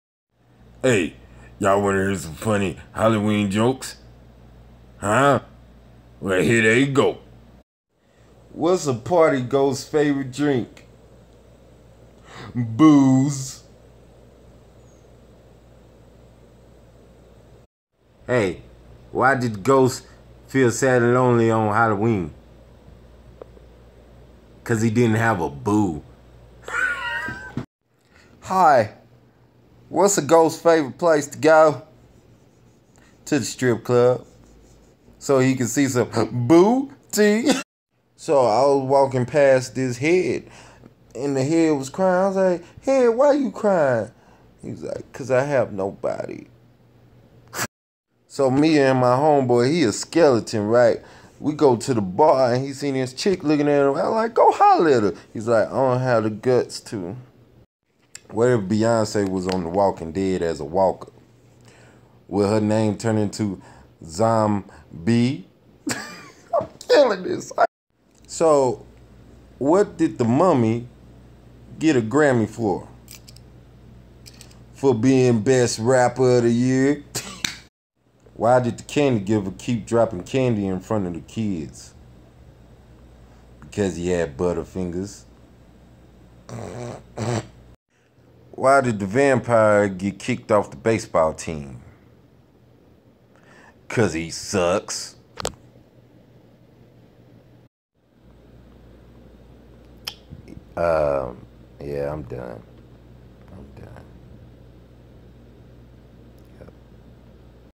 Halloween Dad Jokes (The L.D.)
What better way to celebrate Halloween than with Dad Jokes!